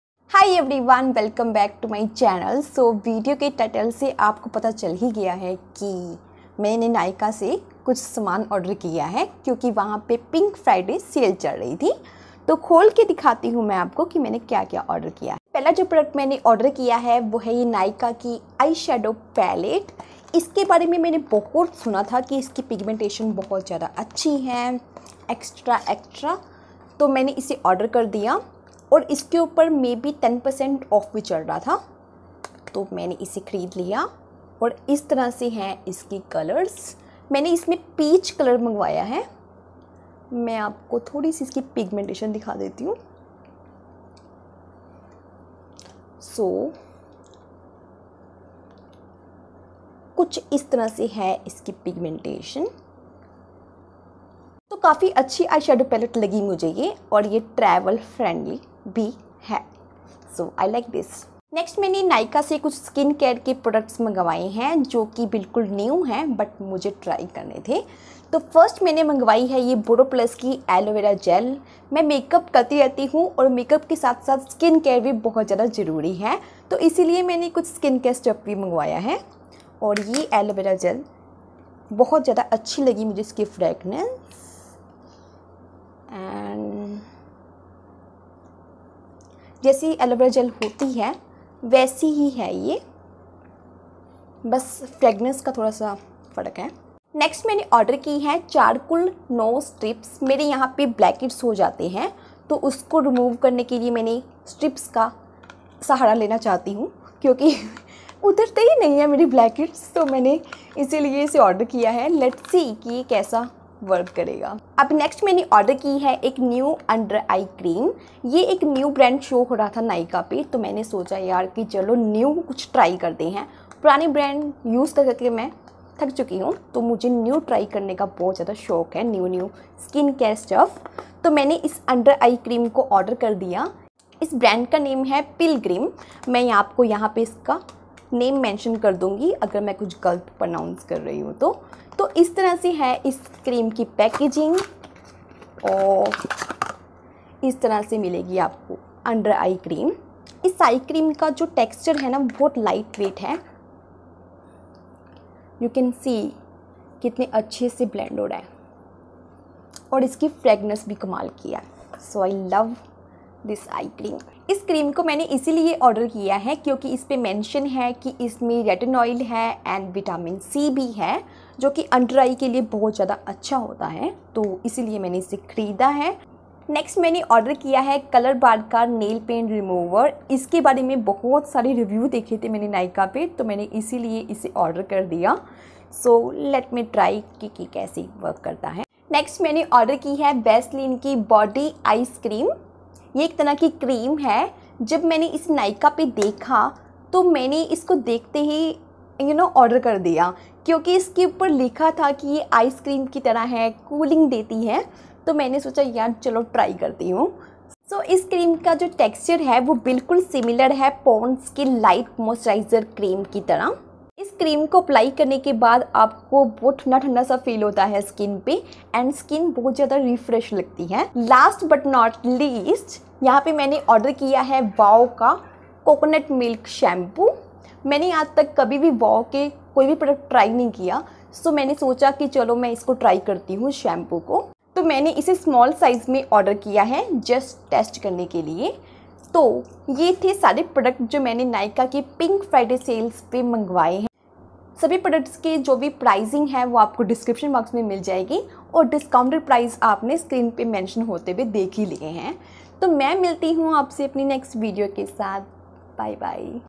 Nykaa Hot Pink Friday Sale Haul 2021|| NEW Skin Care
Hello everyone,

In today's video am showing you Nykaa pink Friday sale haul 2021,I have got lots of new products from Nykaa so I thought let me just share with you.

Nykaa always offers the best cosmetics in offers, This time they have hottestsaleoftheyear so I have got the products from that sale